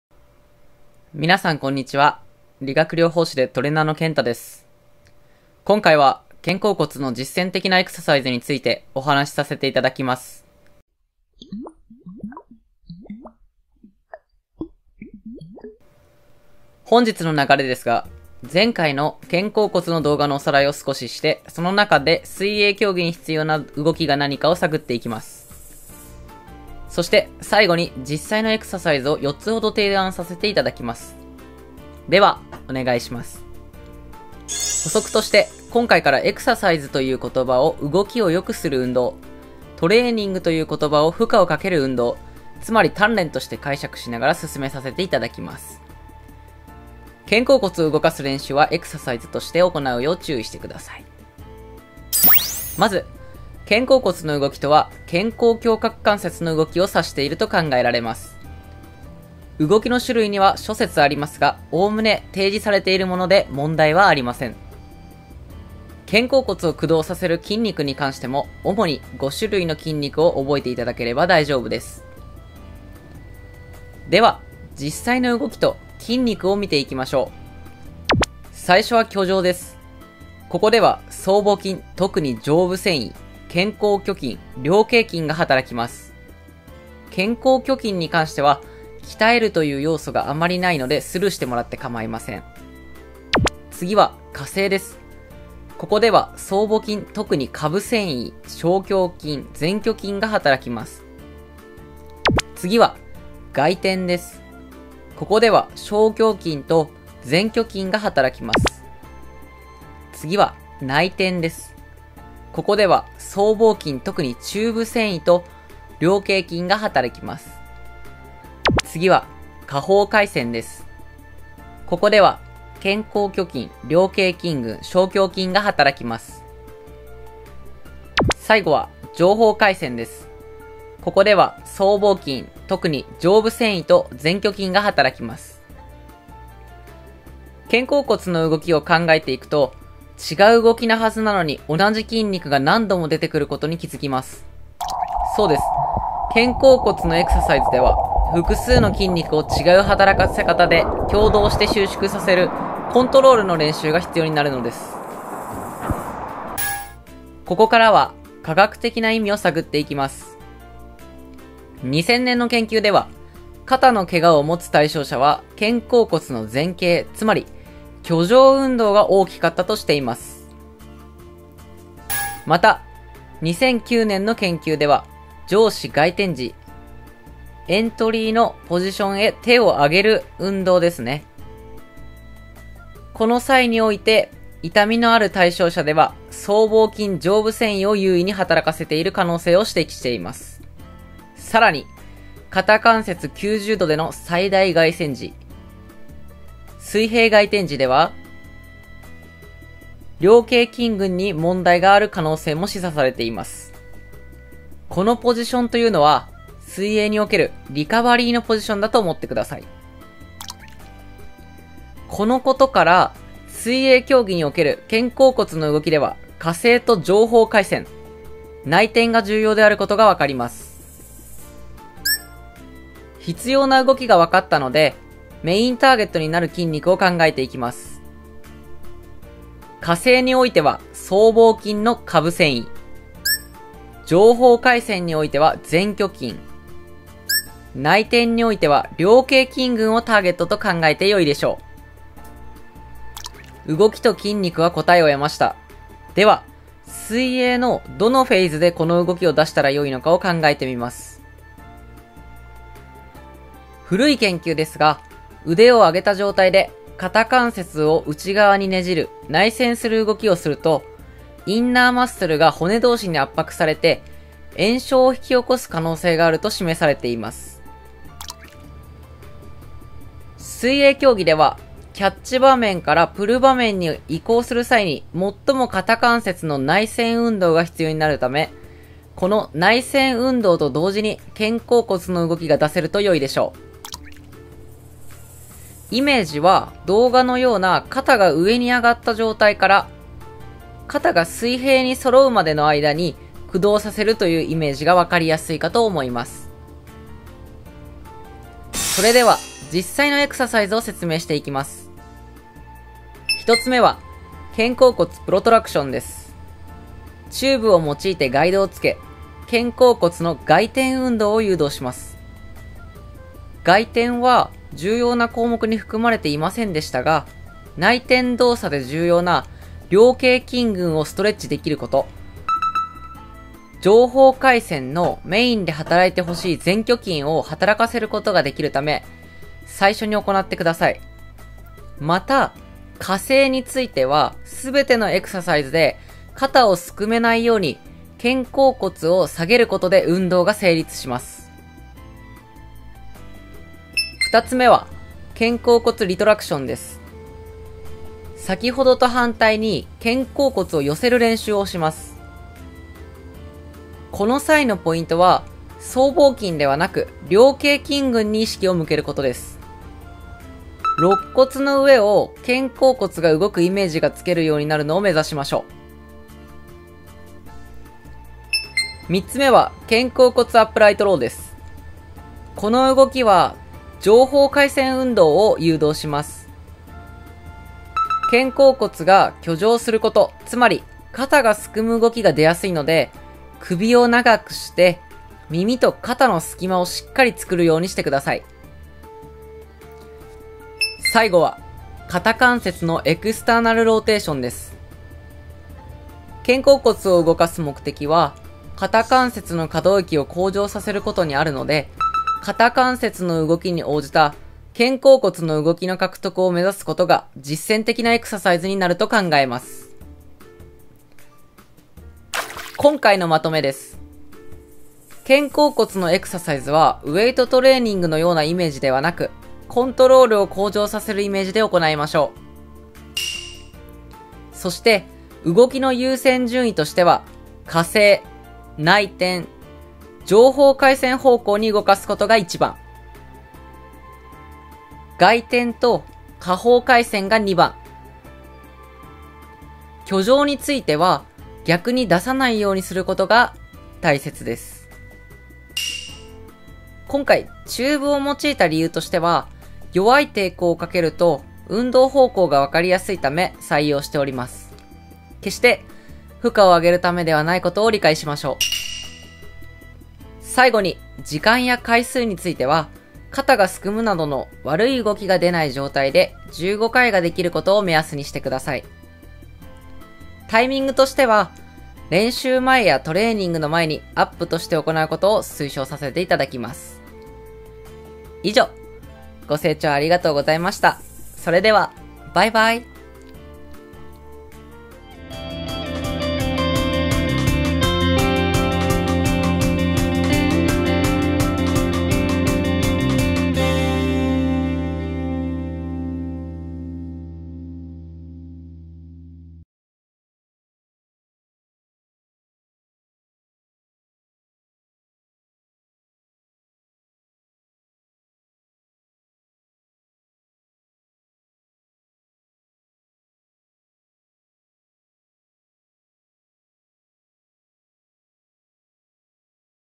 スポーツの専門家が教える「肩甲骨のエクササイズ」とは？
水泳競技において、必要な肩甲骨の動かし方と主要な動きを解説させて頂きました。
他にも数多くのエクササイズがありますが、基本の考え方として、お役に立てれば幸いです。

▶︎筋トレをすると筋肉で体が沈んで泳げなくなるって本当？【トレーナーQ＆A】

▶︎パーソナル指導をご希望の方や案件、広告の依頼はこちらまで